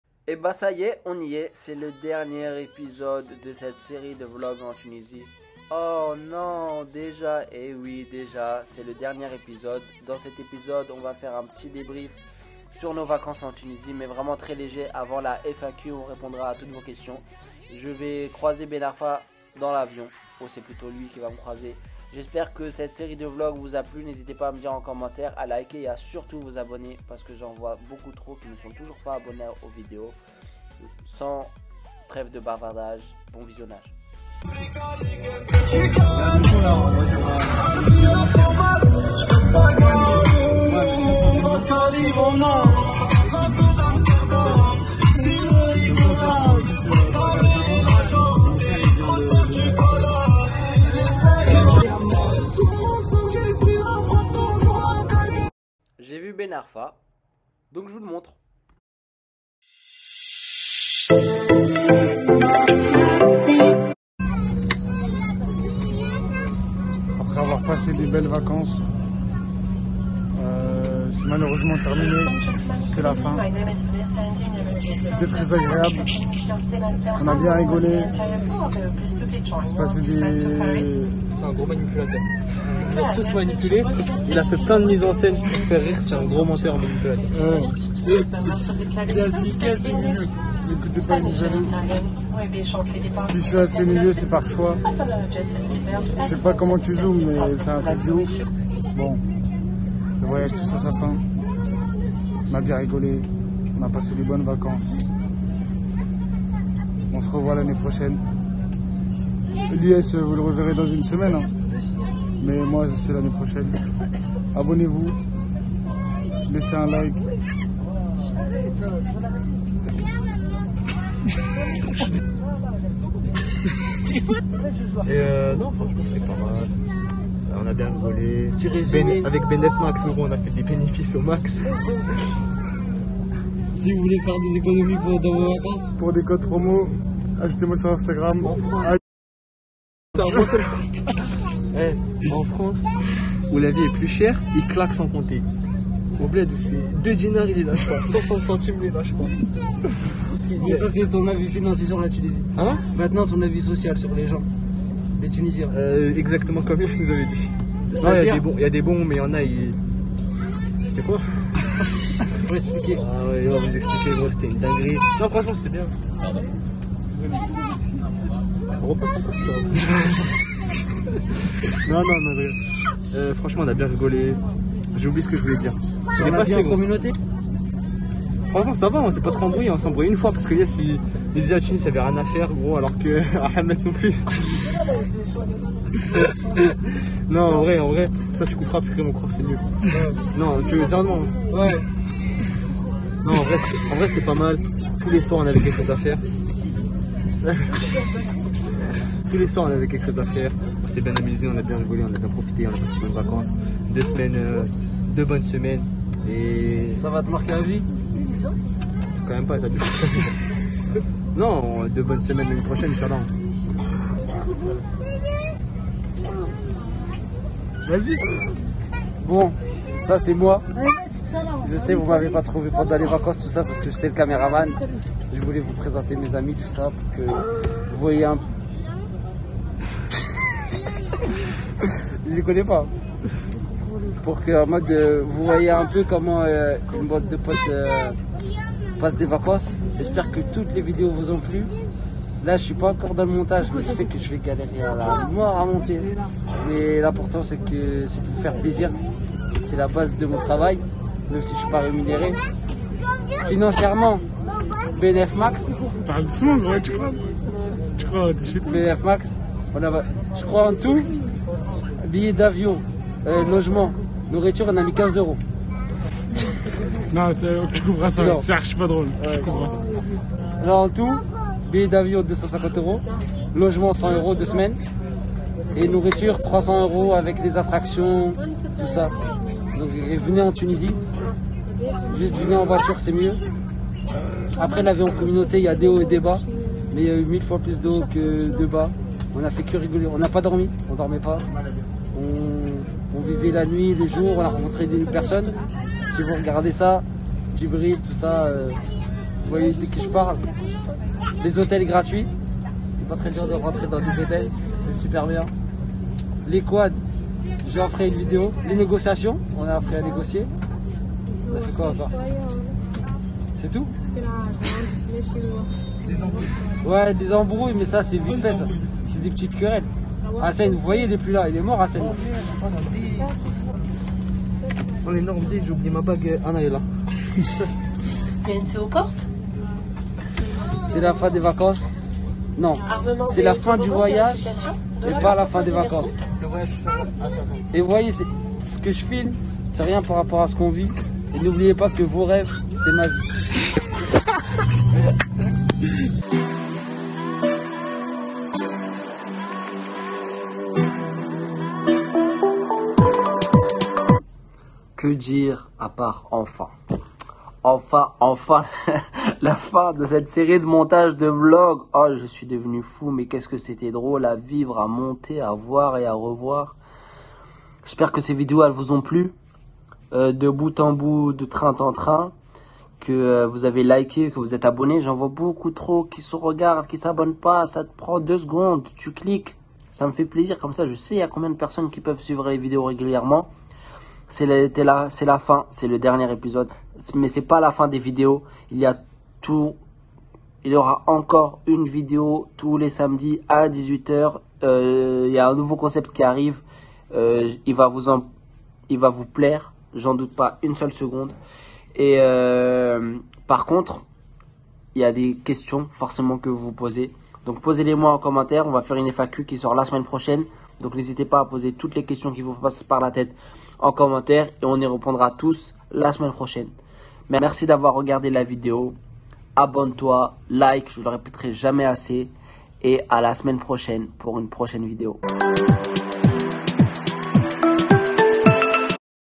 Dernier jour de rêve, on croise Ben Arfa ( VLOG EN TUNISIE)
Des fous rires, des blagues, des bagarres, des chutes et de l’amour

Le début de l’aventure avec 4 amis et moi en Tunisie.

Un épisode tous les samedis à 18h de notre voyage en Tunisie. Il y aura 10 épisodes, et peut-être des bonus si vous êtes sages. 5 mecs dans une maison, vous allez vivre ça comme si vous y étiez.

Merci d’avoir regardé et lu la description. N’hésites pas à liker et partager.